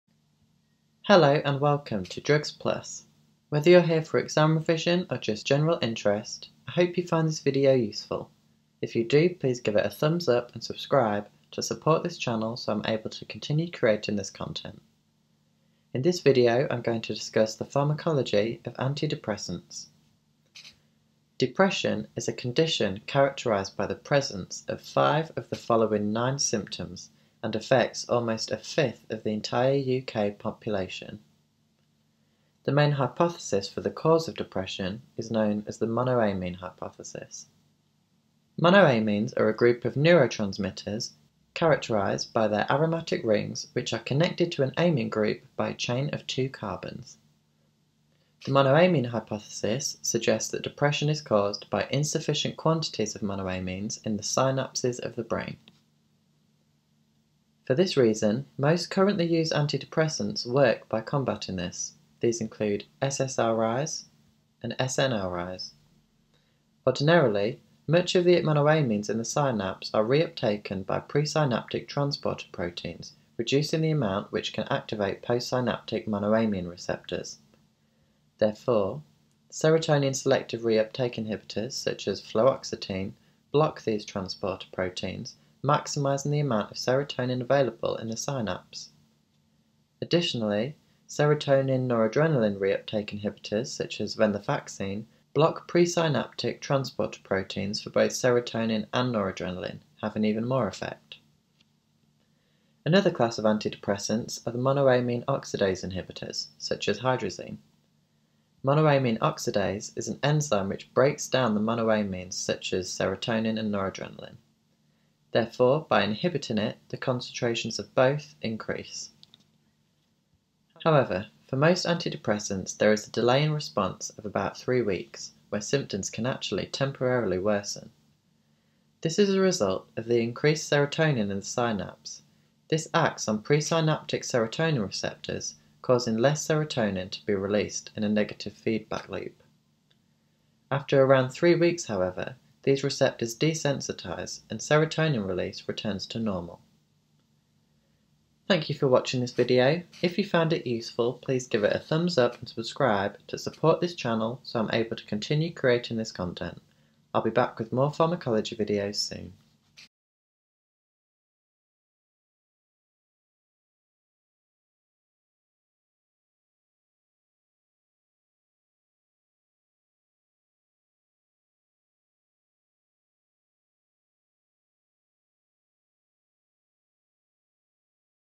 Antidepressants - PROZAC, venlafaxine, etc.
A brief description of the mechanisms of action of some of the most common treatments for depression.